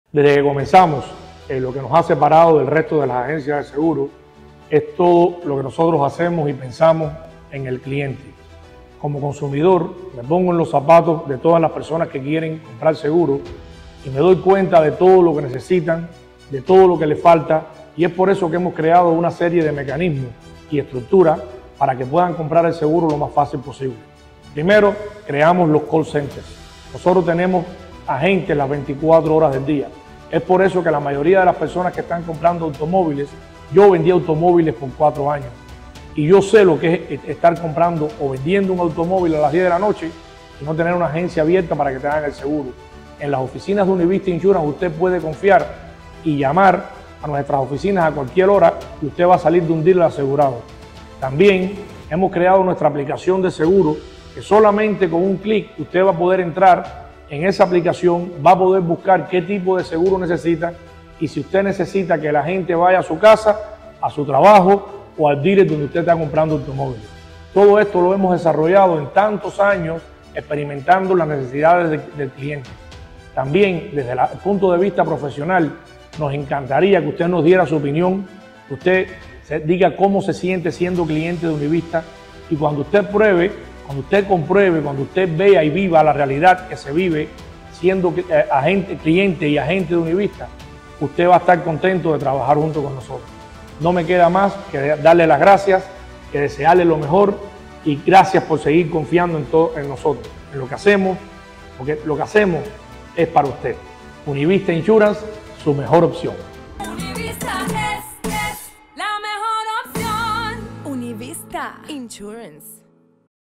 Trabajamos día a día para que nuestros clientes se sientan en las mejores manos
“Hemos desarrollado las herramientas para que nuestros clientes obtengan su seguro de la forma más sencilla posible.” Nuestro Presidente & CEO, Ivan Herrera, nos habla sobre cómo hemos crecido con el paso de los años y cómo trabajamos día a día para que nuestros clientes se sientan en las mejores manos.